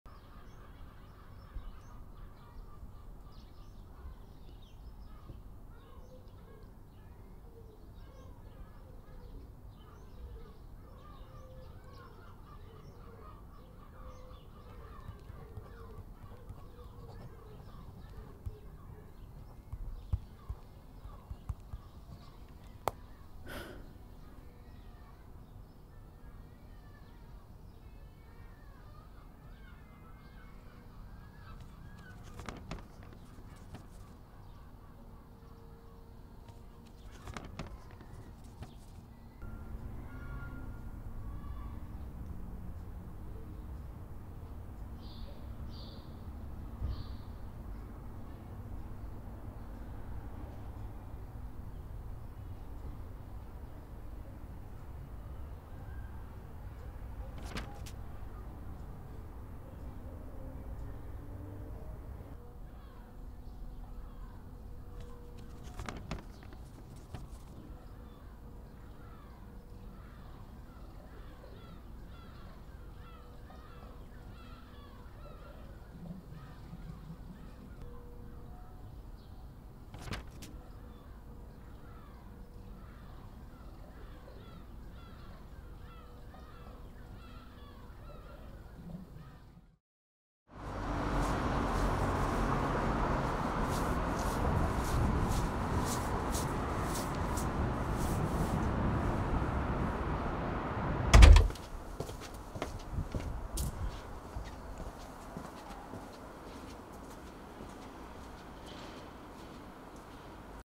Closure
This was the first film I made in my first year of University. The challenge of the assignment was to create a narrative film without any dialogue or music.